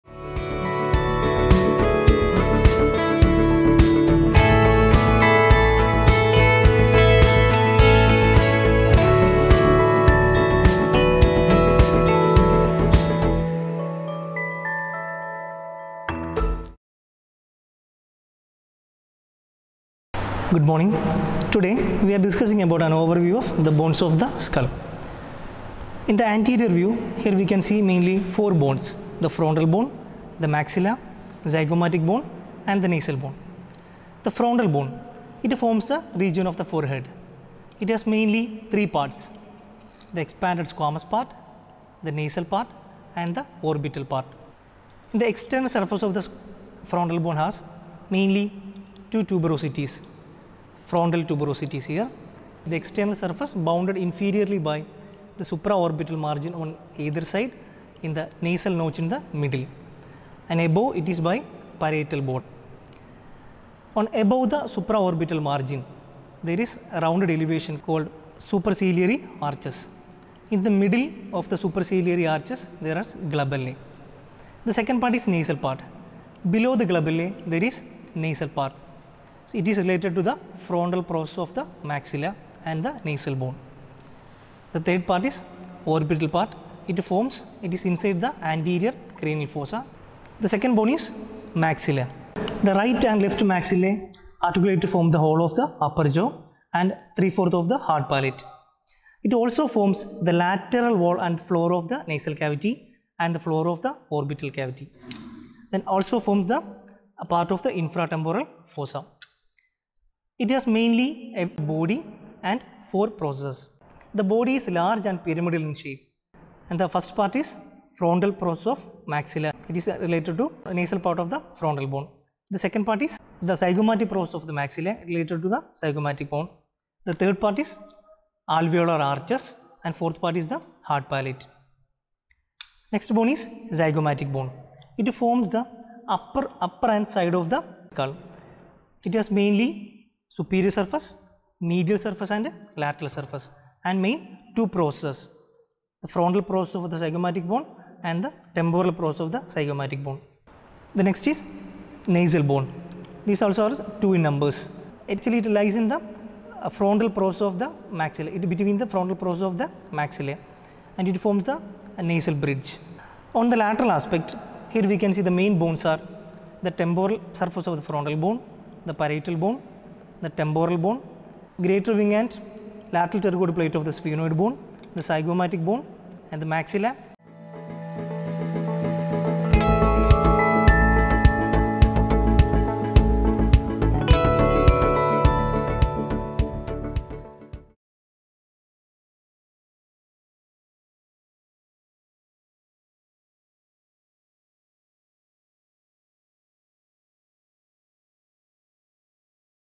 Skull Part-1 Norma Frontalis & Lateralis
Overview of bones of skull in norma frontalis and norma lateralis